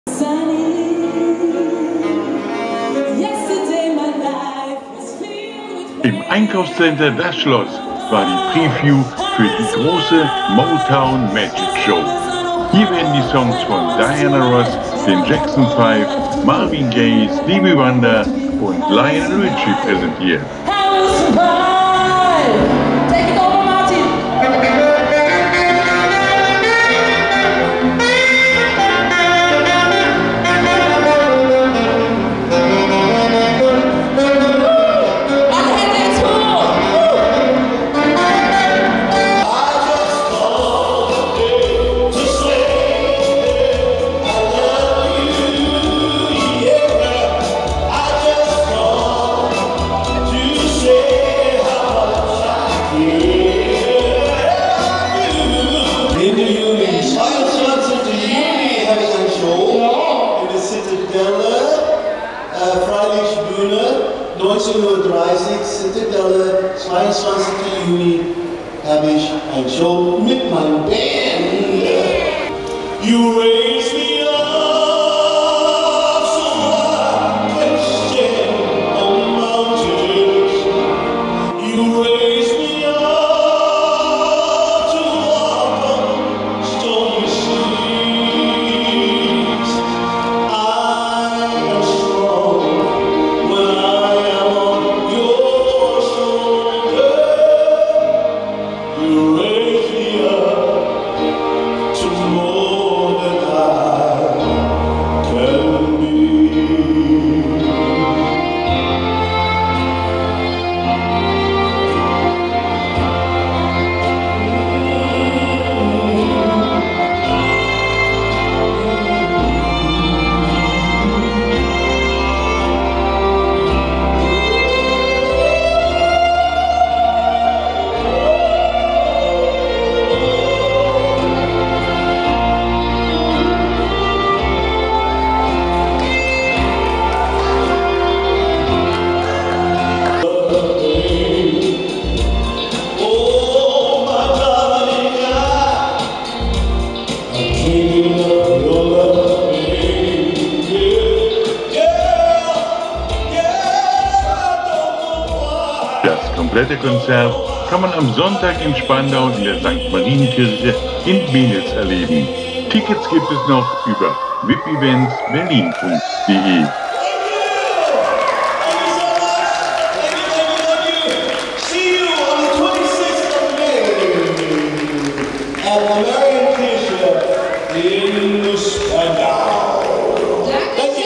MOTOWN MAGIC - Preview im "Das Schloss"
Die Stars von Motown Records in einer magischen Show erleben. Die Musik von Diana Ross & The Supremes, The Temptations, The Four Tops, The Jackson 5, Marvin Gaye, Stevie Wonder und Lionel Richie werden im Konzert von vielen Künstlern präsentiert.